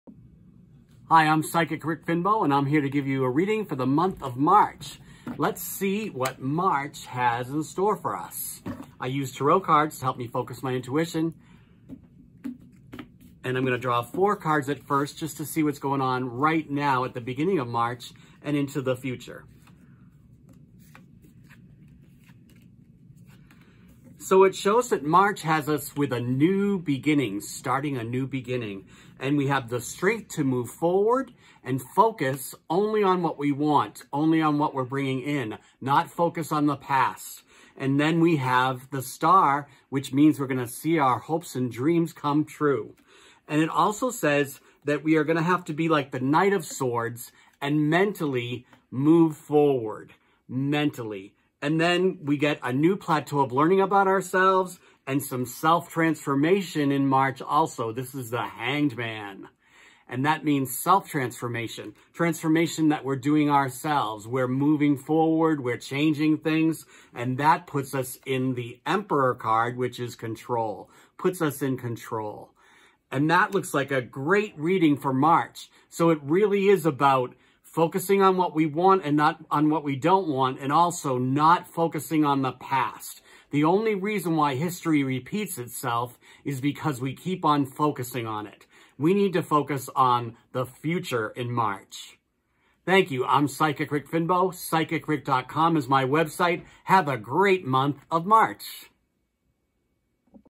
Your Tarot Reading for March 2021
Psychic and Medium Rick Finbow offers you a reading for March 2021.